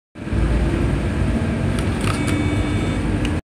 대원여객 370번 2732호 하차벨영상
2020년 7월 7일 촬영

2009년 11월식 대우 BS110CN F/L NGV